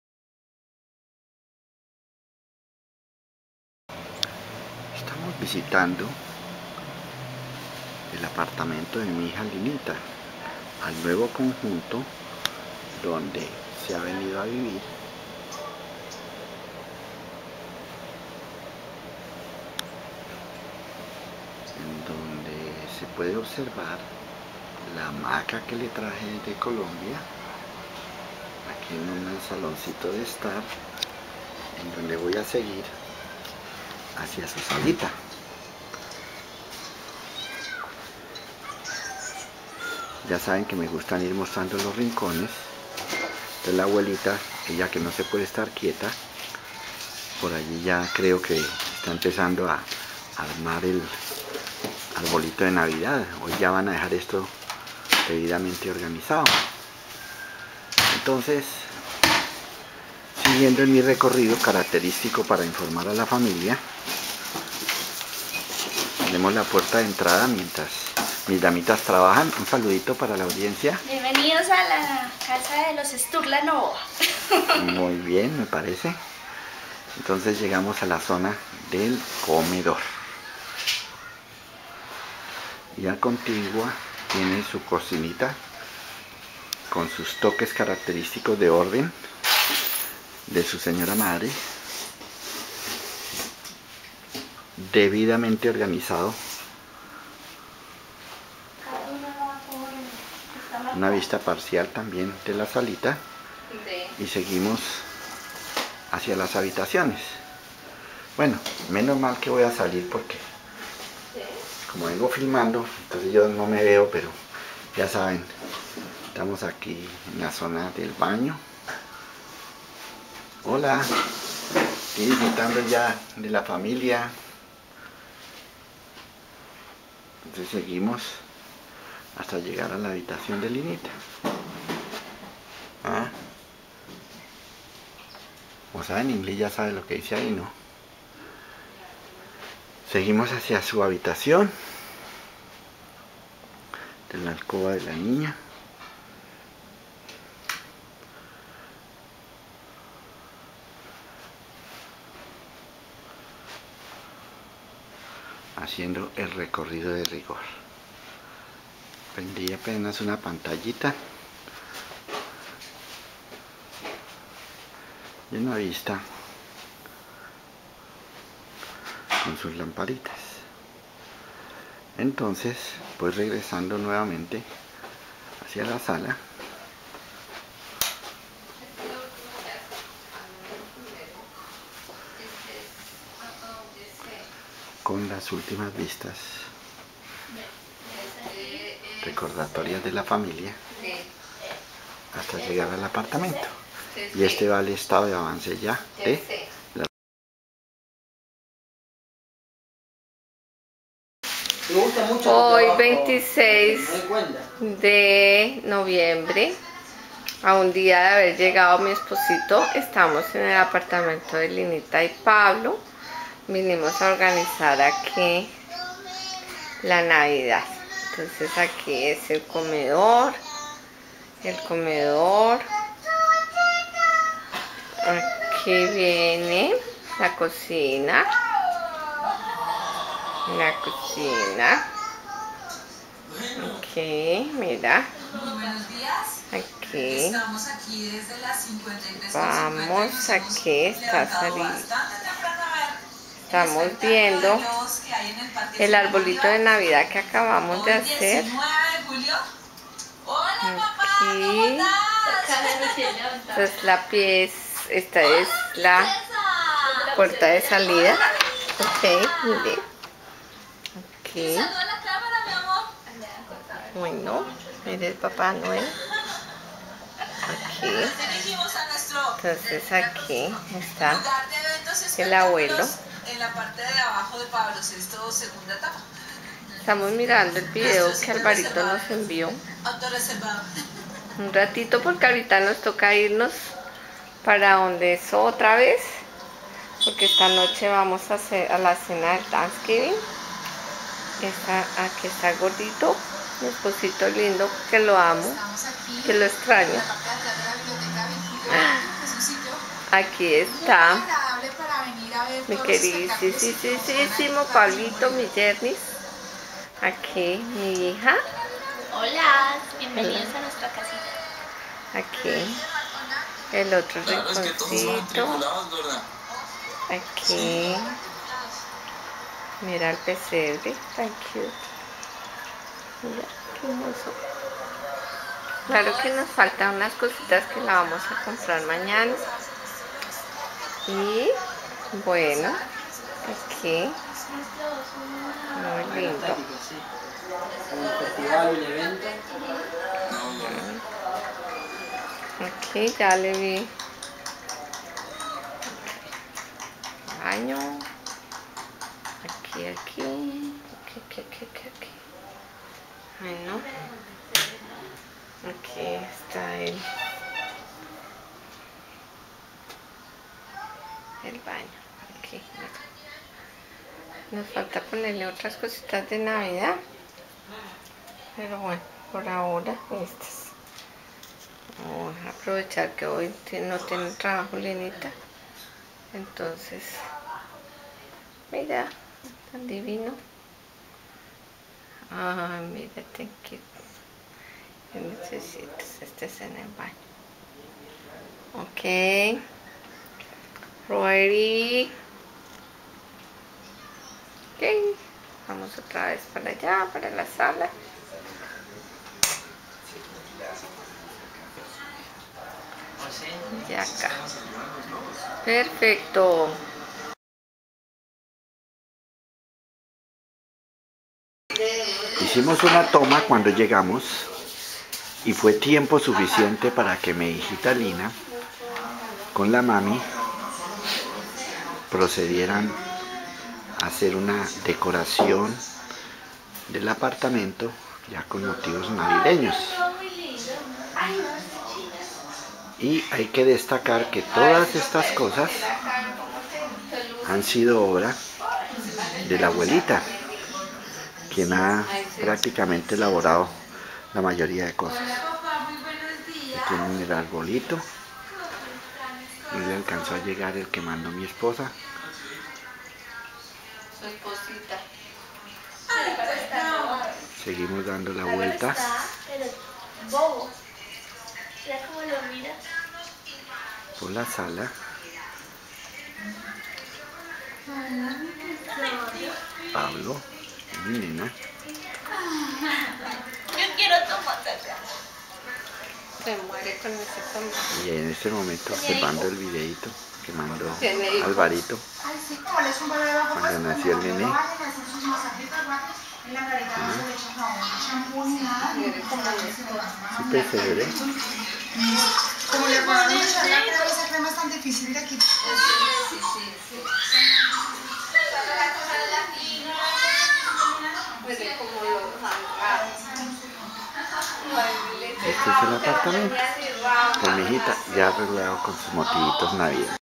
Apartamento Linis y Pablo - Interior
Breve recorrido por el apartamento .. antes y despues de los primeros arreglos de Naviead